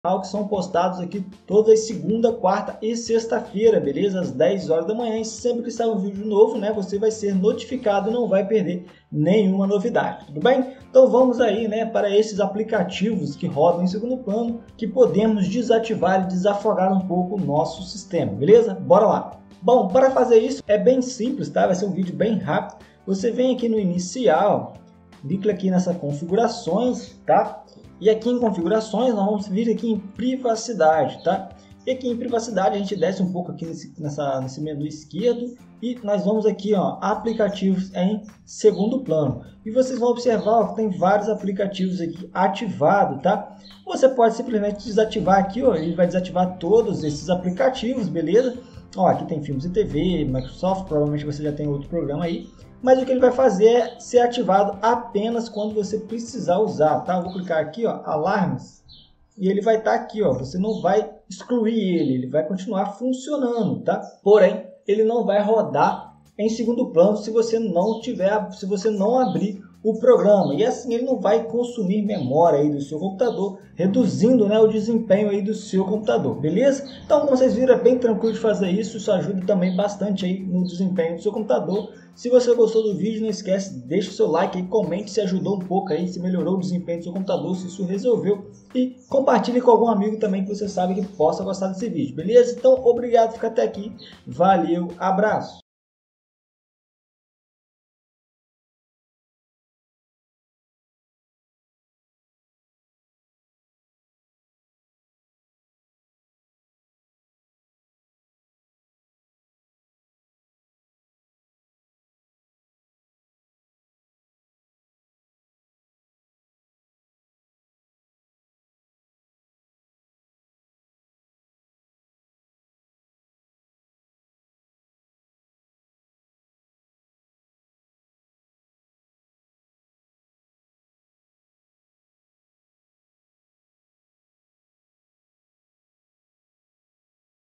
Como desativar aplicativos desnecessários no Windows 10
Como desativar os aplicativos em segundo plano no Windows, e reduzir o consumo de  memória RAM e processador do seu computador, deixando o sistema mais leve e rápido para uso.

Estes aplicativos que ficam rodando sem necessidade acabam também consumindo a bateria do seu notebook, fazendo com que a carga reduza com maior velocidade.

💡 𝗔𝗽𝗼𝗶𝗲 𝗻𝗼𝘀𝘀𝗼 𝗰𝗮𝗻𝗮𝗹:
✓ Compre pelo nosso link na Amazon